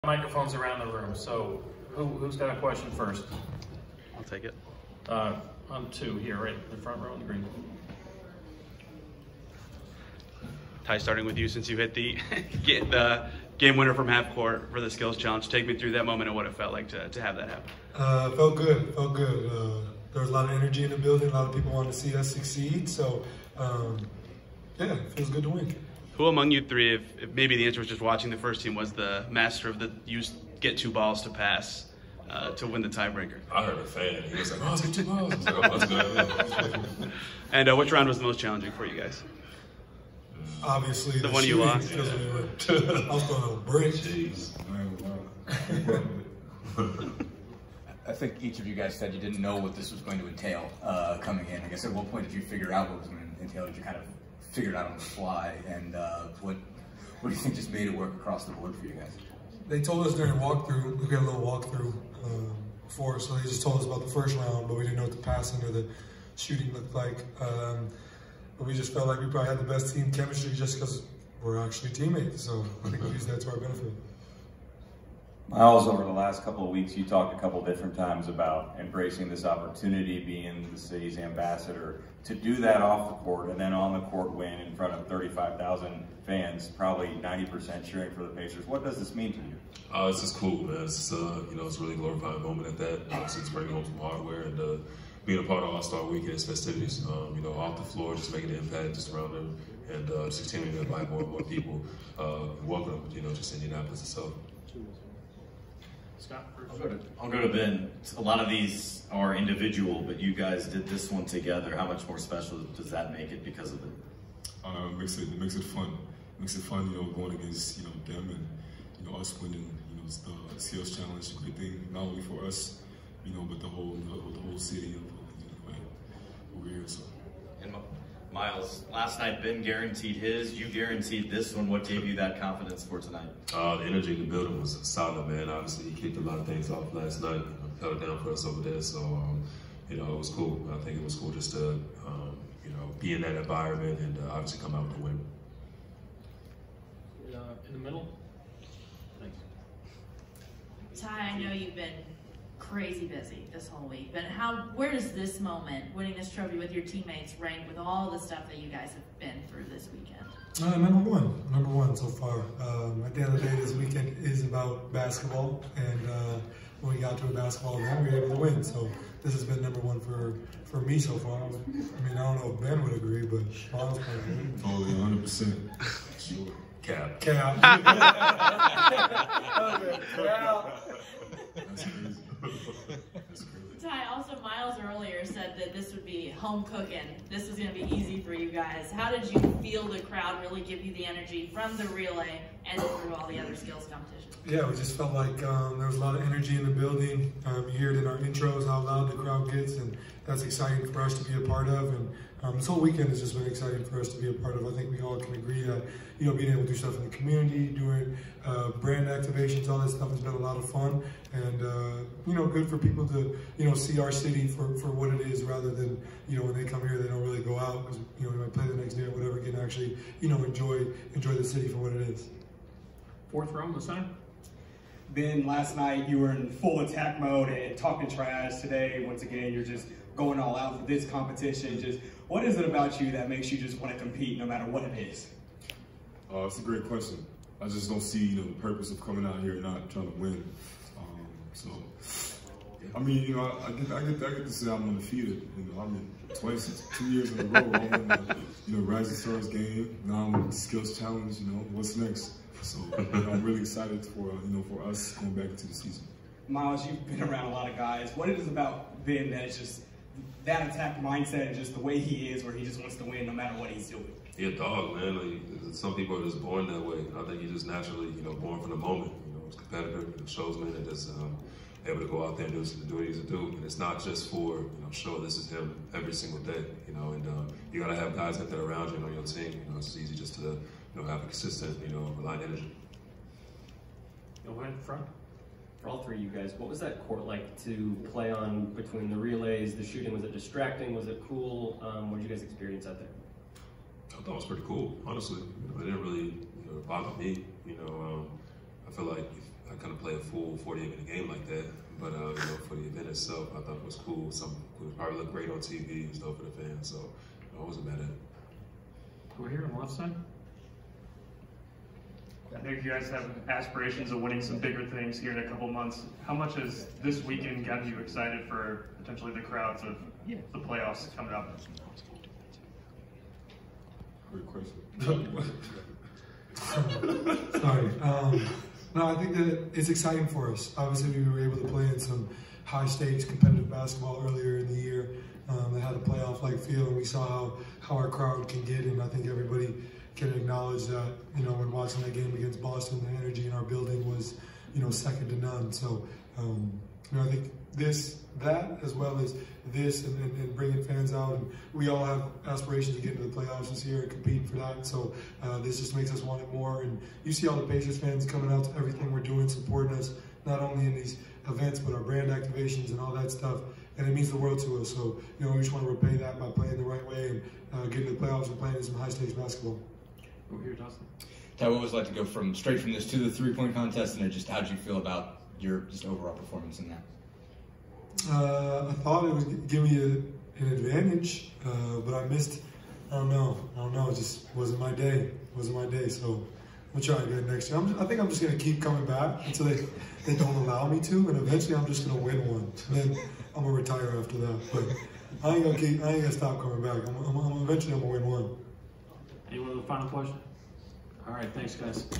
Tyrese Haliburton, Bennedict Mathurin, Myles Turner speak to the media after Skills Challenge win
Indiana Pacers players Tyrese Haliburton, Bennedict Mathurin, and Myles Turner speak to the media after winning the Skills Challenge at 2024 NBA All-Star Weekend.

Locked On NBA League-Wide: Every Team, Fantasy, Draft, WNBA & More